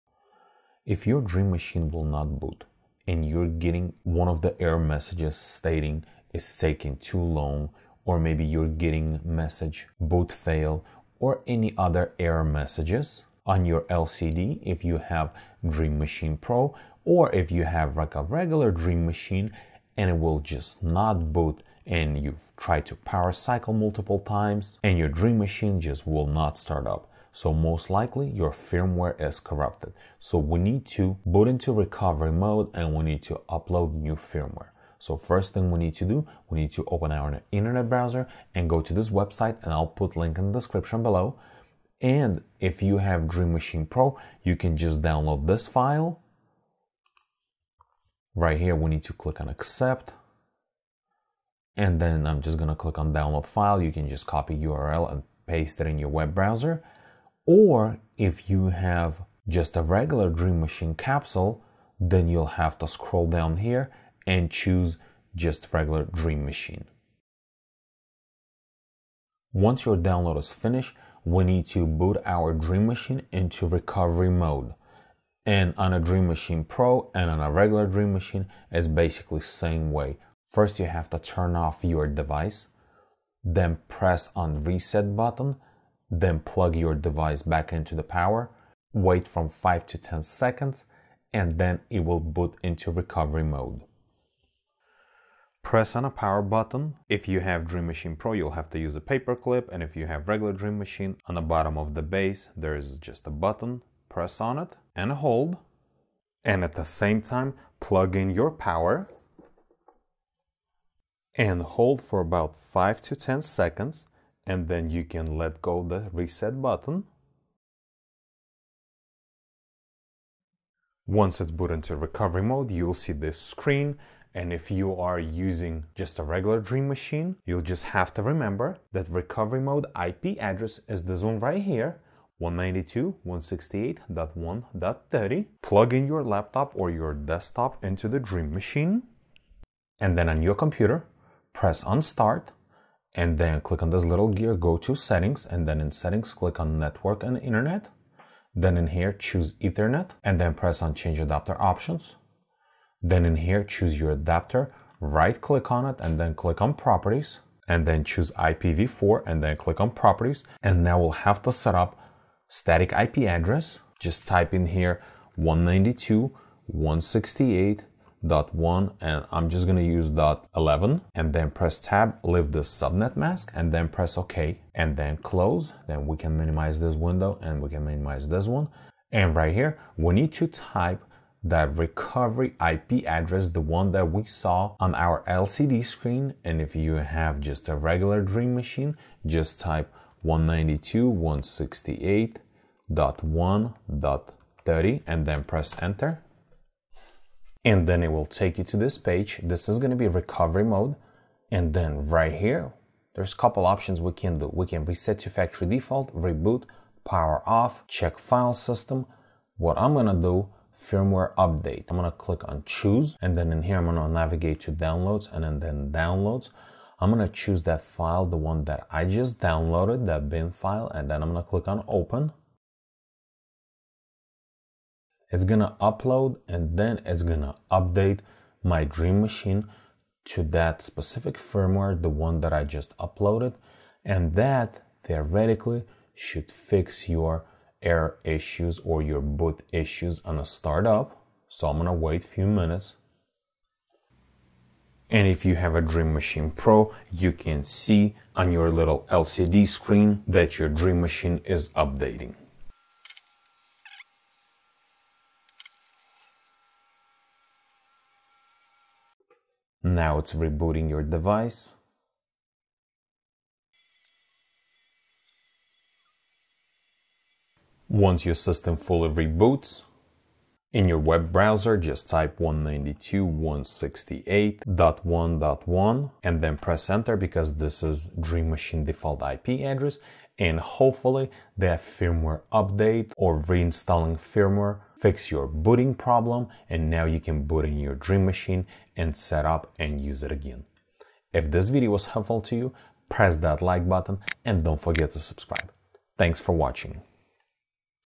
Ubiquiti Dream machine not booting | Quick Fix
If your ubiquiti dream machine is not booting or just malfunctioning or maybe you just would like to downgrade your firmware version I will show you how to manually reload or reset your firmware.

PERSONAL GEAR
4k Asus Monitor
Video Camera Tripod
HDMI Capture Card